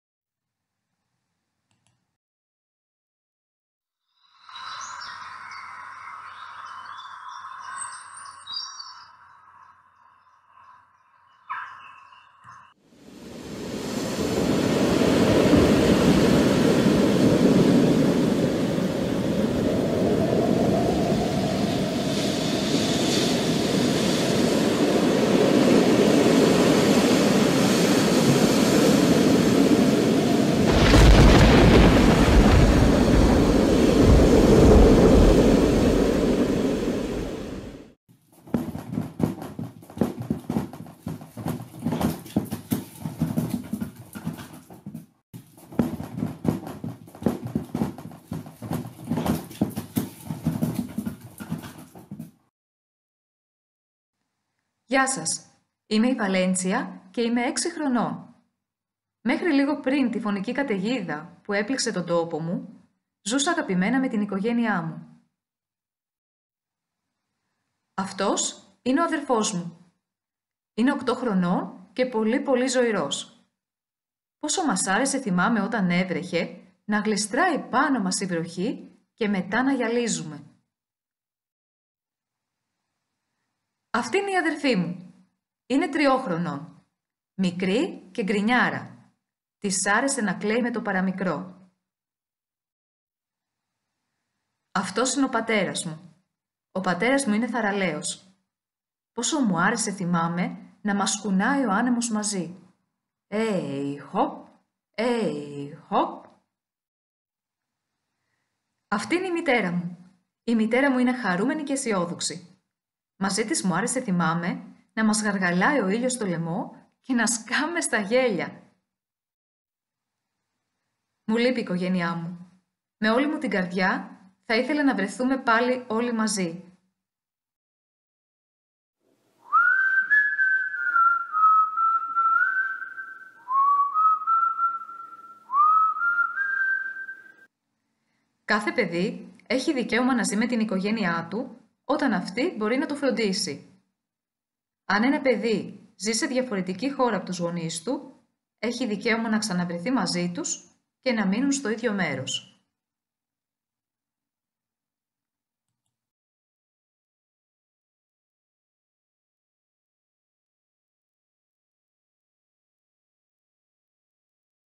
Δικαίωμα στην Οικογένεια | Κέντρο για το Παιδί
Δικαίωμα στην Οικογένεια

Το Κέντρο για το παιδί συνεχίζει την δράση του μέσα από εκπαιδευτικά βίντεο για τα παιδιά.

Πόσο εύκολο είναι να εγκαταλείψεις το σπίτι σου, τον τόπο σου, να χωριστείς από την οικογένειά σου, από τη μια στιγμή στην άλλη;

Ένα βίντεο που αναφέρεται στα Δικαιώματα του Παιδιού, στη σημασία της οικογένειας για την αρμονική ανάπτυξη του παιδιού και στην επανασύνδεση των παιδιών - προσφύγων με τις οικογένειες τους.